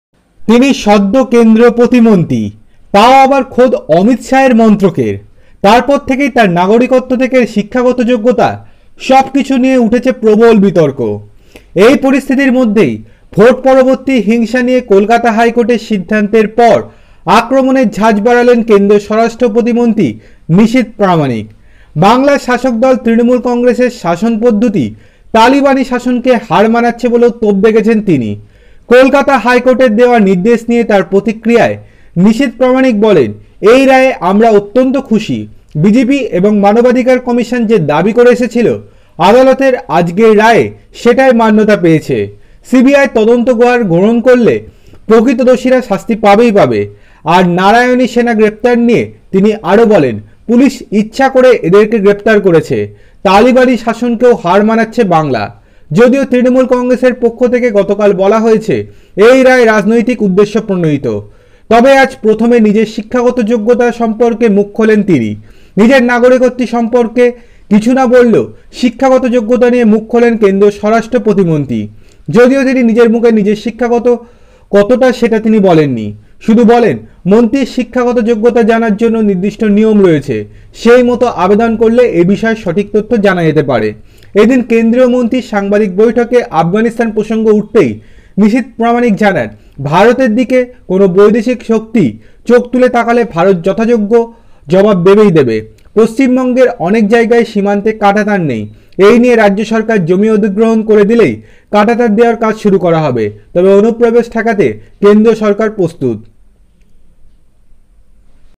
BREAKING- পশ্চিমবঙ্গে তালিবান শাসন চলছে। বিস্ফোরক কেন্দ্রীয় স্বরাষ্ট্র প্রতিমন্ত্রী। মমতাকে ছিড়ল।
BREAKING- পশ্চিমবঙ্গে তালিবান শাসন চলছে। বিস্ফোরক কেন্দ্রীয় স্বরাষ্ট্র প্রতিমন্ত্রী। মমতাকে ছিড়ে খেয়ে বিস্ফোরক।

westbengalelection2021exit polls
bidhansava election result 2021